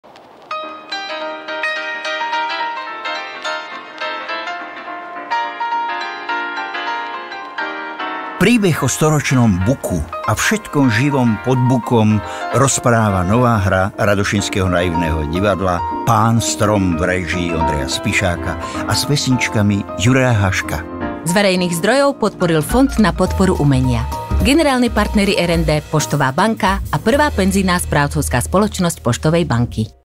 Pán Strom (2021)
Autor hry a textov piesní: Stanislav Štepka
Réžia: Ondrej Spišák
Hudba: Juraj Haško
Kostýmy: Katarína Hollá
Scéna: František Lipták
Dramaturgička: Darina Abrahámová
Choreografia: Ladislav Cmorej
Producent: Ladislav Hubáček
„Príbeh o živej drevine“ – taký podtitul dal Stanislav Štepka nevšednému javiskovému dramatickému rozprávaniu o človeku a prírode v poetickej komédii Pán Strom. Totiž na scéne divákom farbisto porozpráva svoj životný a mimoriadne pútavý príbeh – inak na prvý pohľad mlčanlivý storočný strom – buk. Pútavo pohovorí o všetkom všednom aj nevšednom, veselom i smutnom, poetickom i tragickom, ale hlavne ľudskom, čo sa za tých predchádzajúcich sto rokov dialo na ňom, pod ním a s ním.
Rozhovorí sa nielen strom a ľudia naokolo (dedinská rodina, hasiči, poľovníci, samovrah, židovské dievča atď.), ale aj lastovičky, lykožrúty, jelene, ďatle, sova, líška, jazvec, sláviky, vlk, zajac atď.), slovom všetko nadmieru živé. A hádam každý, kto chce podať svoju správu o pestrom, mnohorakom, ale aj inteligentom a nevyspytateľnom živote v lese.
Zároveň s príťažlivým životným príbehom stromu a lesa nepriamo sledujeme aj storočné pestré udalosti ľudí a spoločnosti a  prichádzame či nepriamo dospejeme k poznaniu, že stromy, príroda, flóra a fauna tvoria humanitnú jednotu a žijú s ľuďmi v neoddeliteľnom spoločenstve, v ktorom sa musíme naučiť spolu žiť a navzájom sa rešpektovať.
Nevšedná, poetická hra Stanislava Štepku ponúka veľa nečakaných a prekvapujúcich humorných situácií a nepriamo aj smeruje k morálnemu apelu.
Premiéry boli 3. a 4. júna 2021.